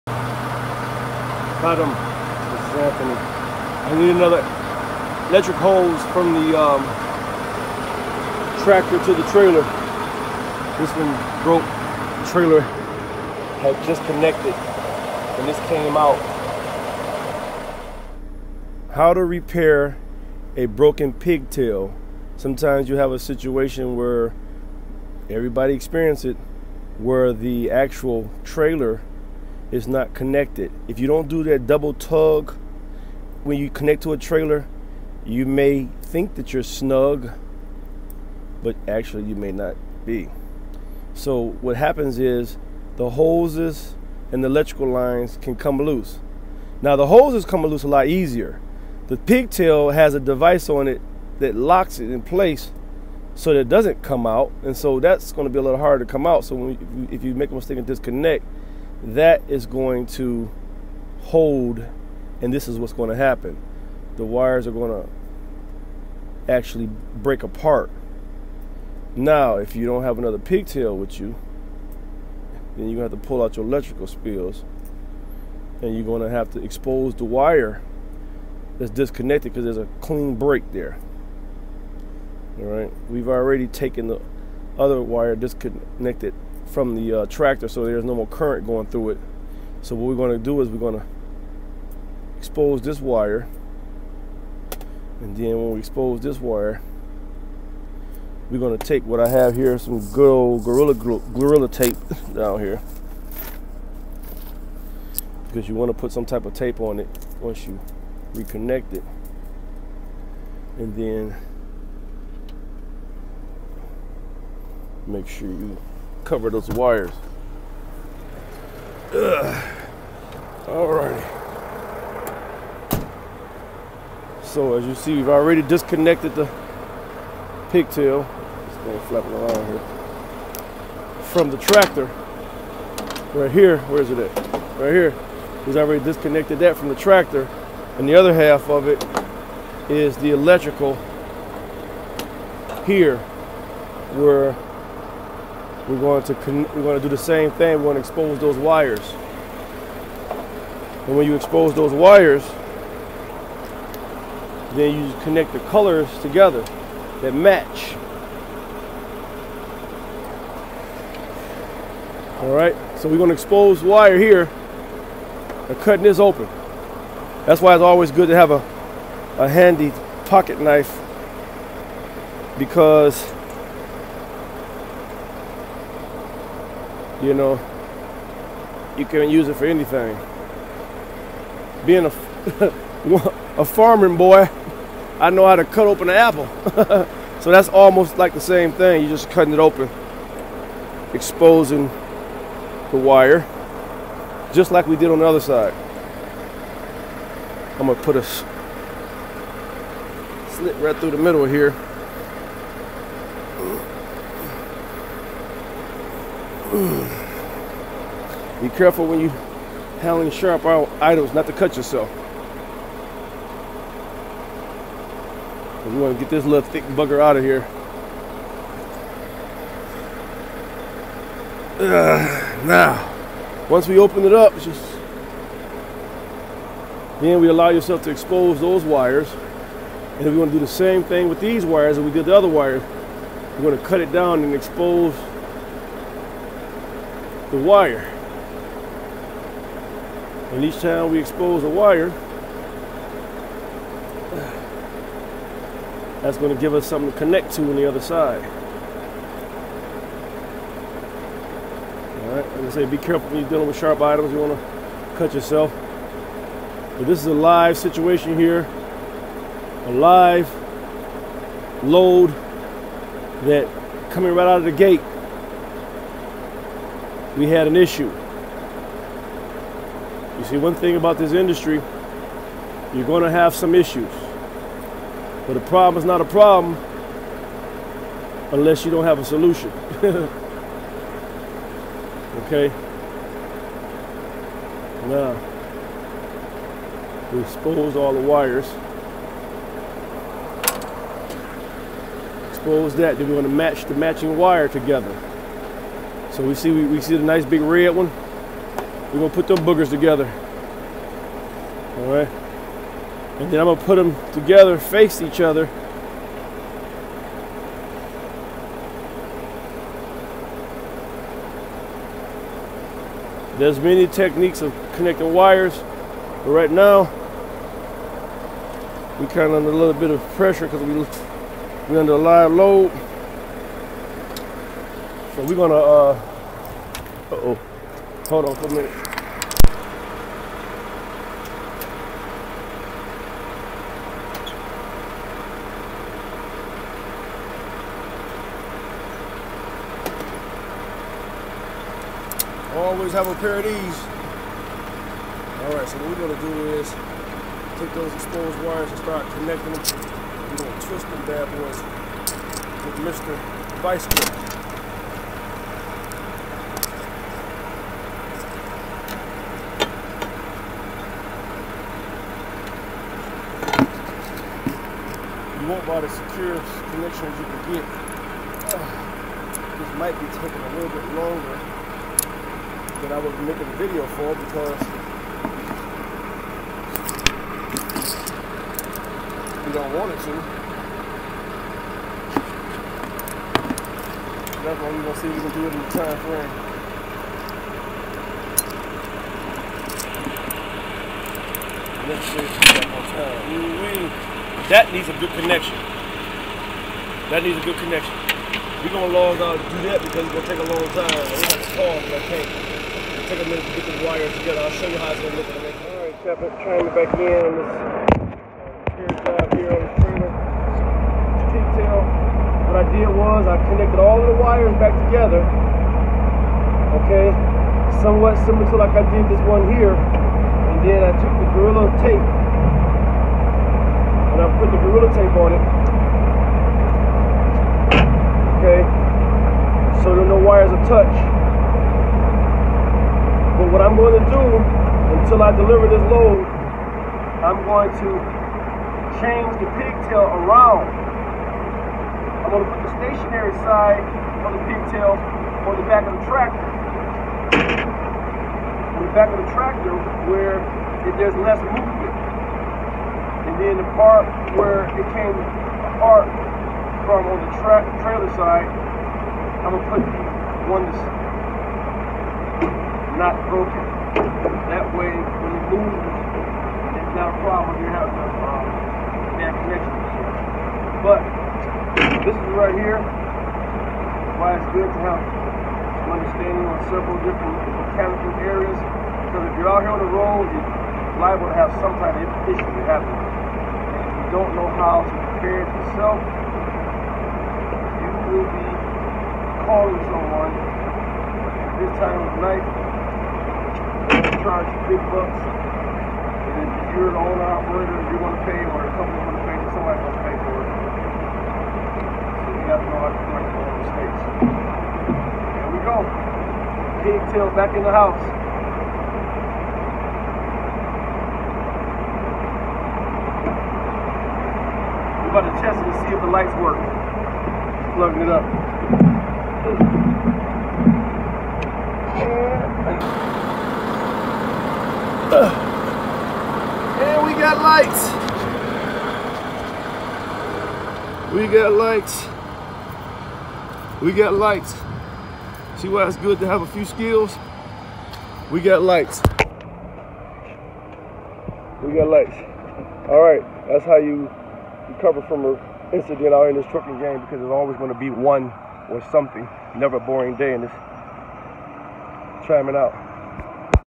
How to repair a electric line on a tractor trailer
This video talks about how to repair electric line on a tractor trailer better known as the pigtail